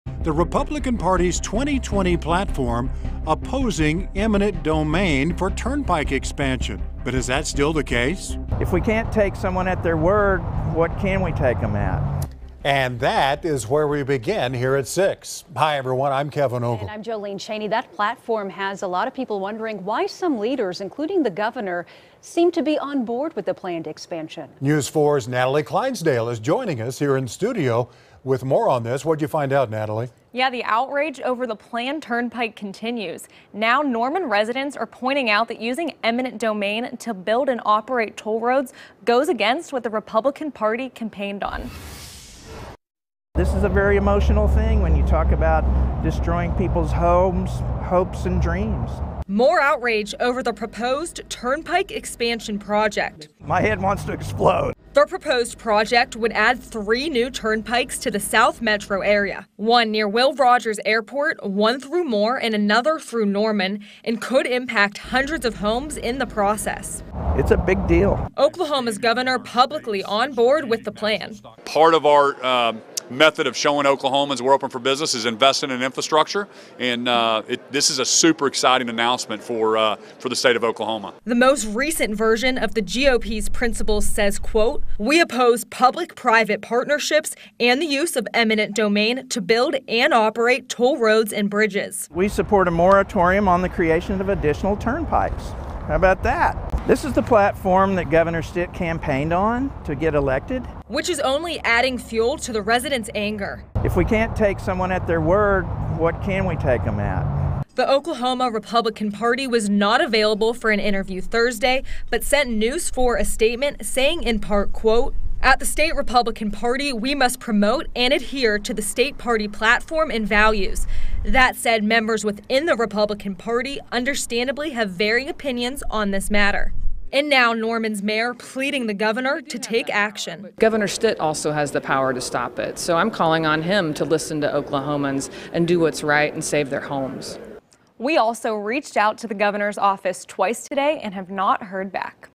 Norman residents argue Republican platform contradicts proposed turnpike expansion
Outrage over the proposed turnpike expansion project continues.